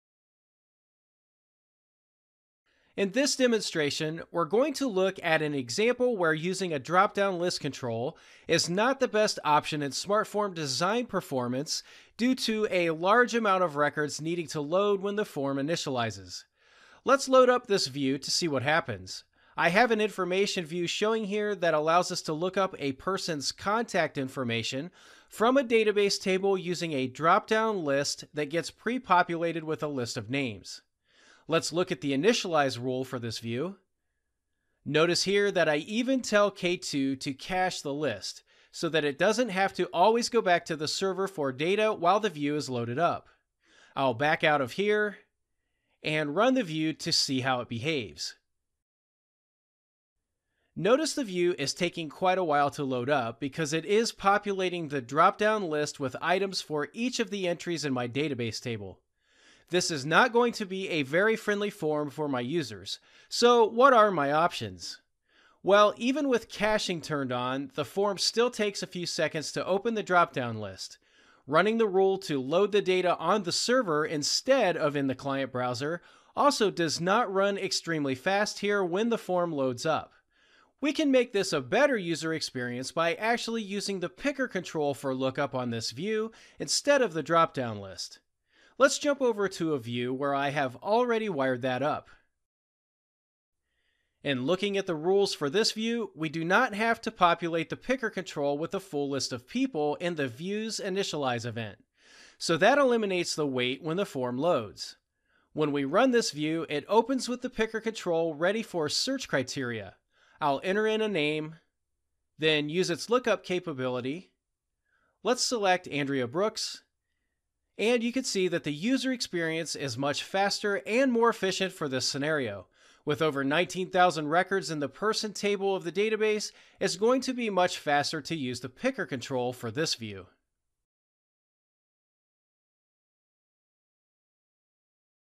Picker vs Drop-Down List
In this demonstration, we’re going to look at an example where using a drop-down list control is not the best option in SmartForm design performance due to a large amount of records needing to load when the form initializes. This is part of our IT Developer training course.